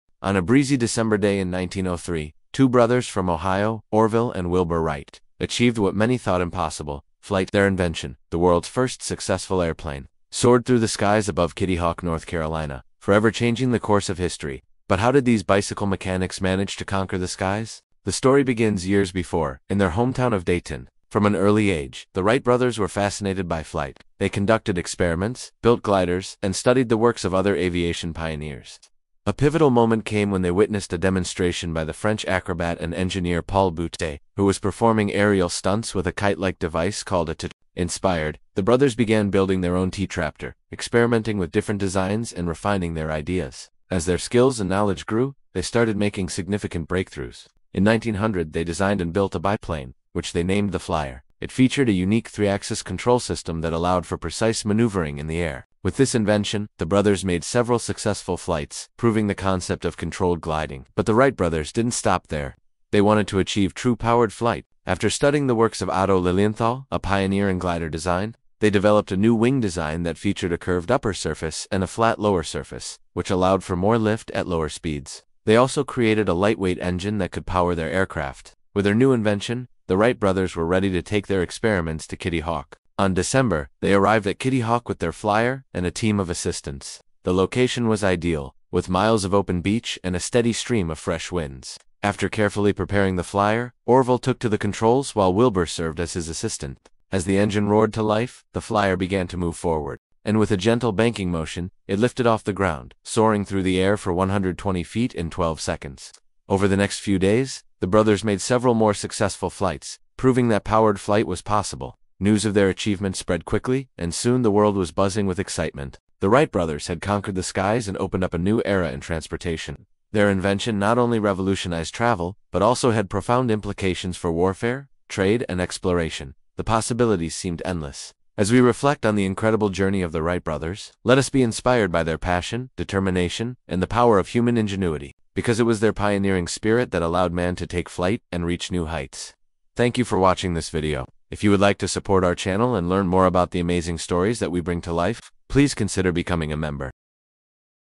The Birth of Flight: How the Wright Brothers Conquered the Skies at Kitty Hawk
On December 17, 1903, history was made when Orville and Wilbur Wright achieved the first powered, controlled, and sustained flight in Kitty Hawk, North Carolina. 🚀✈️

In this video, we’ll explore:
🌬️ Why Kitty Hawk’s winds and dunes made it the perfect testing ground.
🔧 How two bicycle builders engineered the first successful flying machine.
💡 The trial-and-error process behind the Wright Flyer’s wings, engine, and controls.
🌍 How their invention reshaped transportation, warfare, and human imagination forever.

The Wright Brothers didn’t just fly—they proved that human ingenuity could overcome gravity itself.